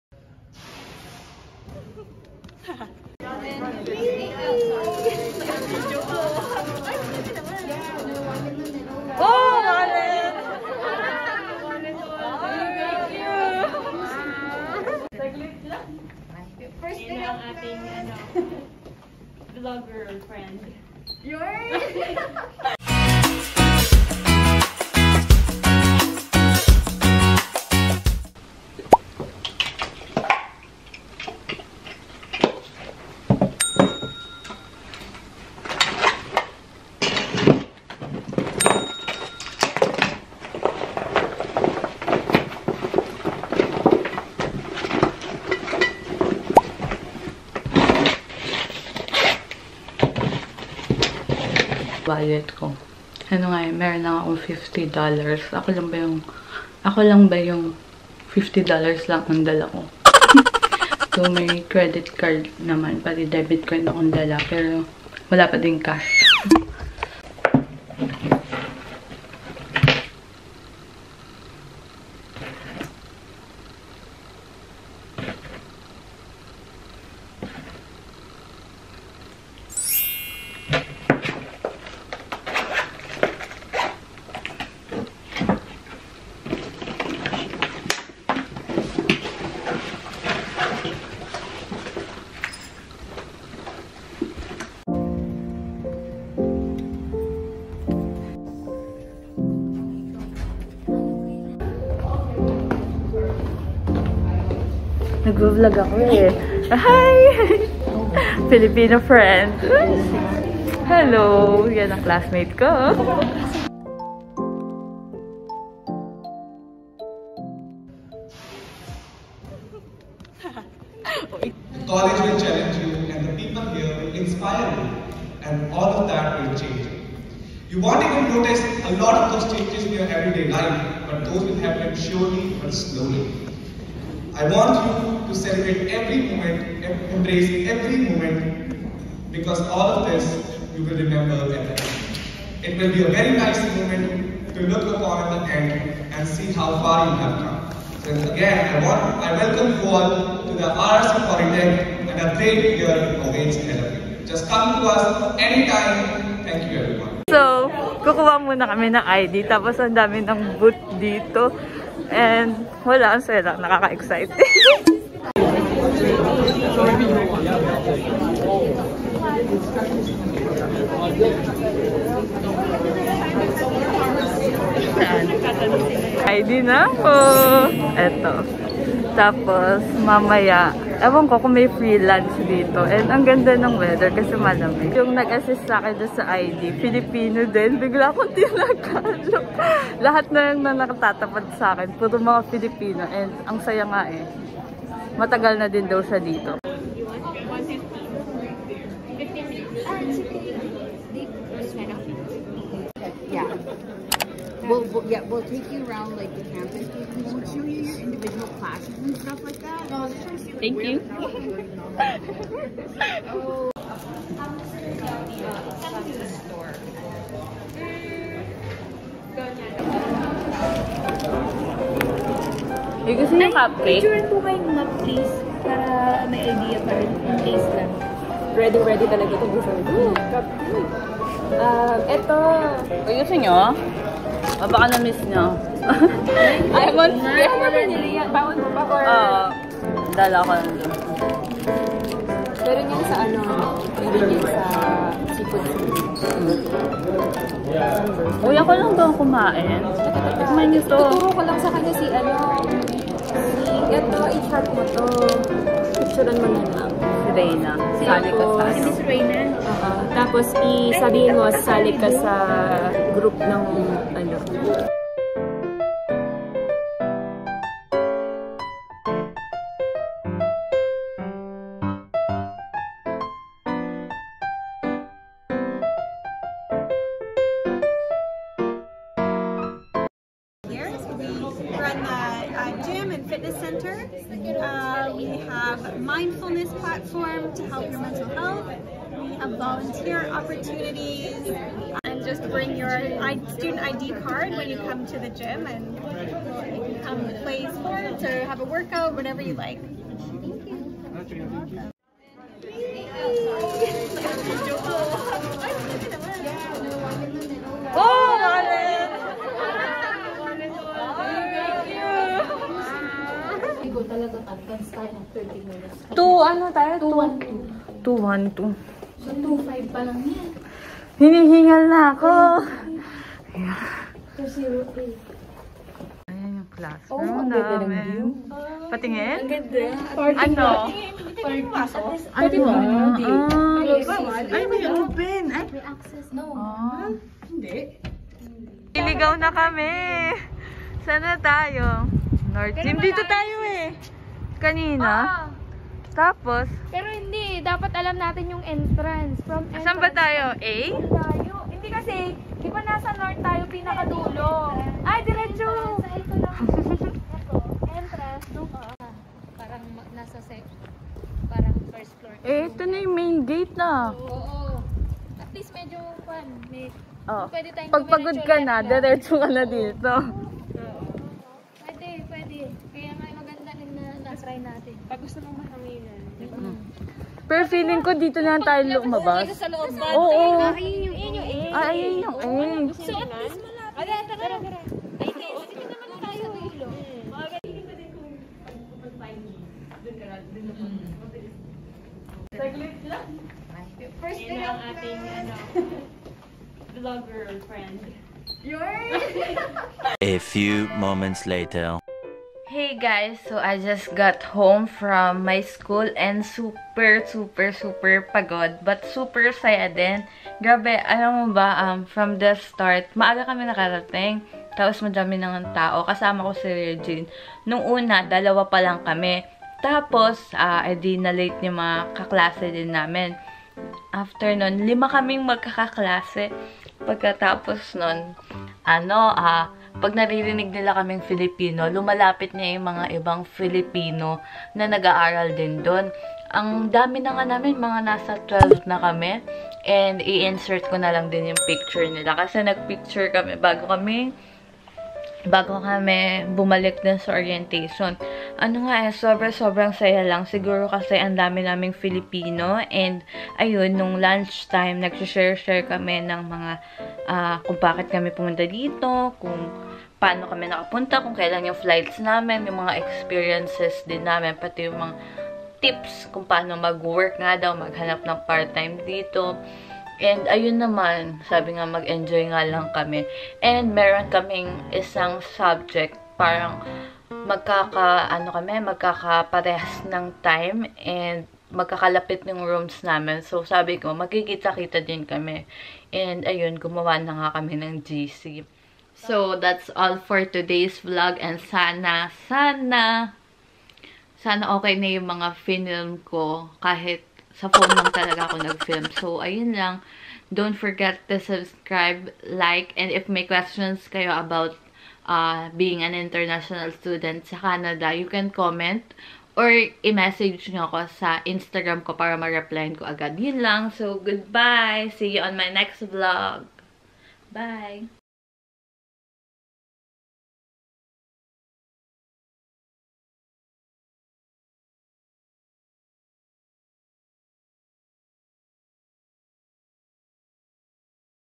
ORIENTATION DAY AT RED RIVER COLLEGE | FILIPINO INTERNATIONAL STUDENT IN CANADA
Today's video is all about my orientation day where I also met some Filipinos studying at Red River College. I hope you'll enjoy.